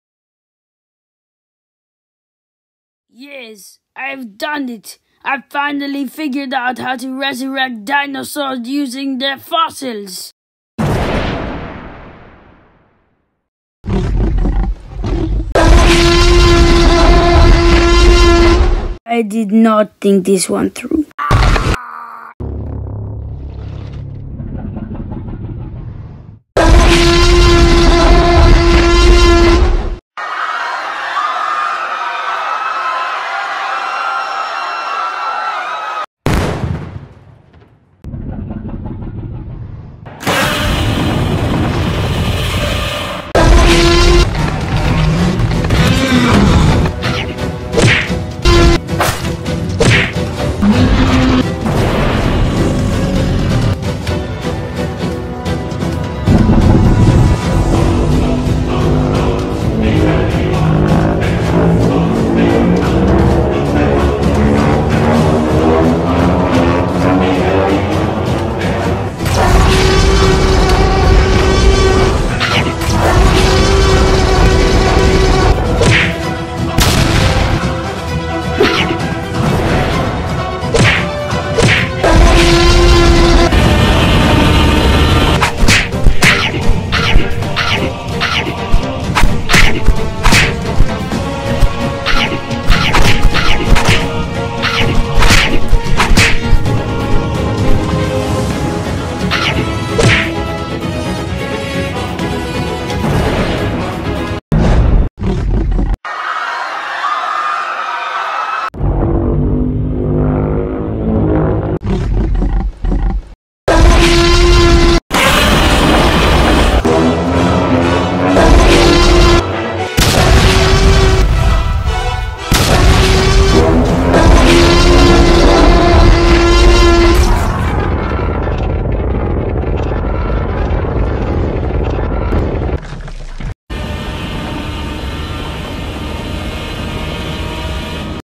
Godzilla vs T Rex (GodzillaGamer Battles)
After a mad scientist resurrects a T Rex using its fossil, it breaks free and goes on a rampage in the city! But don’t worry, Godzilla is here to restore natural balance.